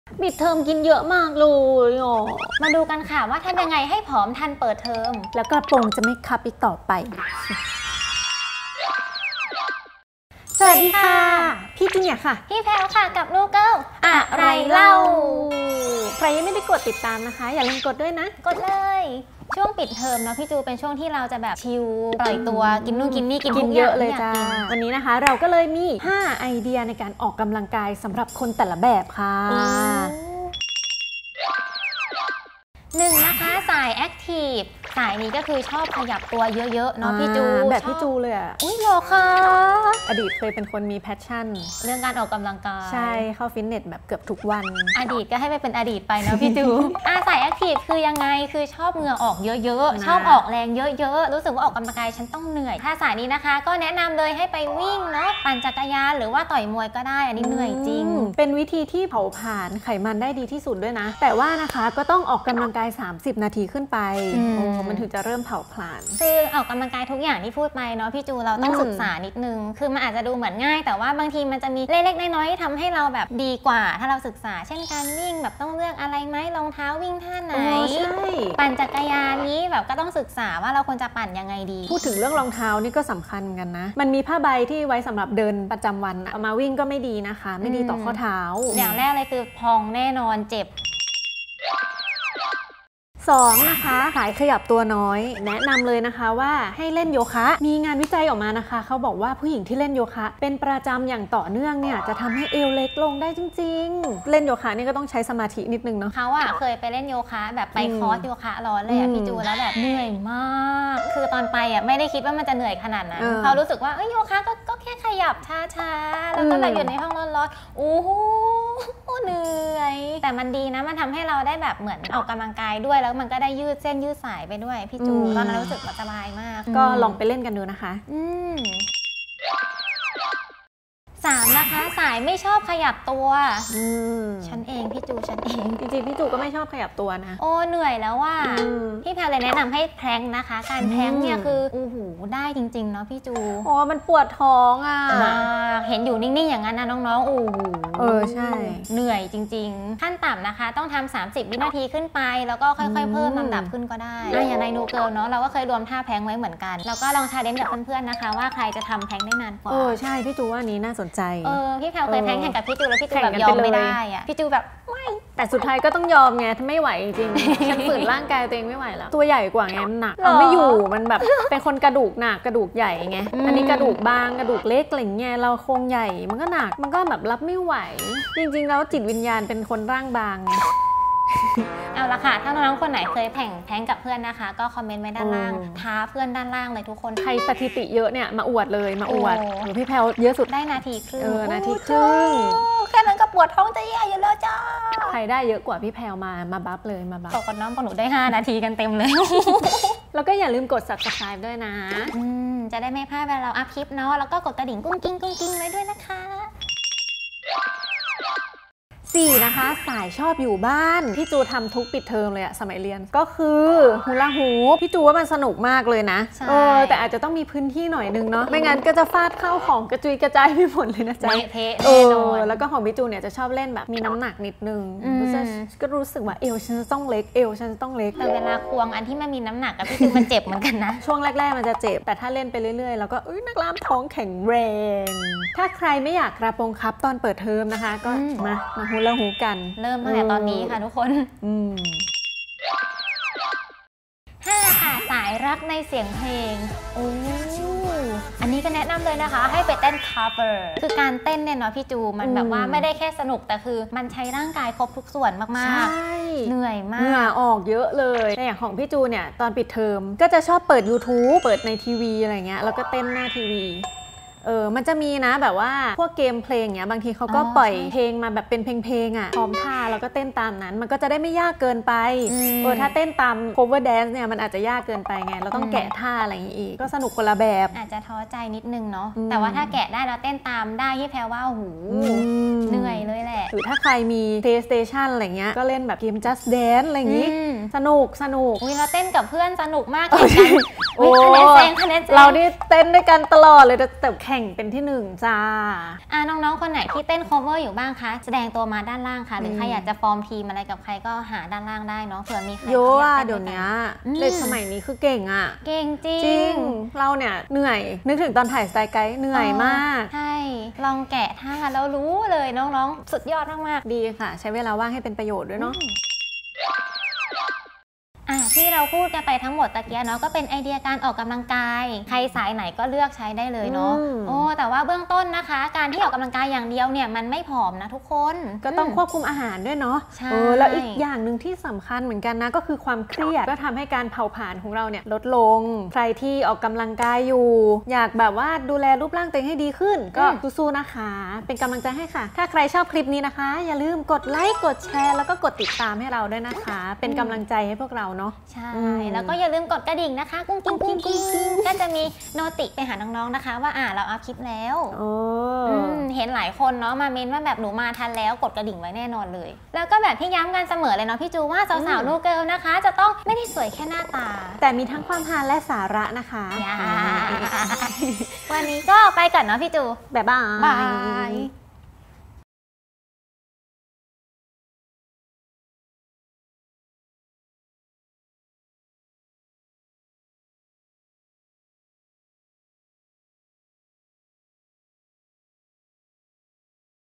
เปิดเทอมผอมแน่! 5 ไอเดียออกกําลังกายปิดเทอมสำหรับคนแต่ละสาย | NUGIRL อะไรเล่า
ปิดเทอมคือช่วงเวลาแห่งการชิล นอนอืด ดูซีรี่ส์ กินขนม กินทุกอย่างที่อยากกิน เผลอแป๊บเดียวเท่านั้นแหล่ะ น้ำหนักขึ้นมาหลายกิโล พุงขึ้นมาอีกหลายนิ้ว! เอาล่ะ กระโปรงนักเรียนก็คับจนใส่ไม่ได้แล้ว NUGIRL อะไรเล่าคลิปนี้ พี่จูเนียร์ พี่แพรวจะมาช่วยทุกคนลดน้ำหนักในช่วงปิดเทอมกัน กับ 5 วิธีออกกำลังกายสำหรับคนแต่แบบ ปิดเทอมนี้เราจะมาออกกำลังกายลดพุงไปพร้อมๆ กันนะคะ เปิดเทอมกระโปรงเราจะไม่คับอีกต่อไปป

5 ไอเดียออกกำลังกายสำหรับคน 5 สาย
0:32  สายแอคทีฟ ชอบเหงื่อออก ชอบเหนื่อย ออกแรงเยอะๆ แนะนำให้ออกกำลังกายด้วยการวิ่ง ปั่นจักรยาน ต่อยมวย
1:33  สายขยับตัวน้อย แนะนำให้เล่นโยคะ
2:11  สายไม่ชอบขยับตัว แนะนำให้แพลงค์ (Plank)
3:28  สายชอบอยู่บ้าน แนะนำ ฮูลาฮูป
4:04  สายรักในเสียงเพลง แนะนำการเต้นโคฟเวอร์ (Cover Dance)

อ่านบทความเต็มๆ
"5 วิธีลดเอวให้เล็กทันใจ เปิดเทอมไม่ต้องซื้อกระโปรงใหม่แล้ว"
- ทำยังไงให้ลดความอ้วนได้
- หั่นมื้อหลักออกเป็นมื้อย่อยช่วยลดความอ้วน
- เลือกกินให้มากขึ้น
- หาวิธีออกกำลังกายที่เหมาะกับตัวเอง

อย่าลืมอัพเดททุกเรื่องไลฟ์สไตล์ของสาววัยรุ่นได้ที่

Instagram พี่จูเนียร์ : Juniorjoo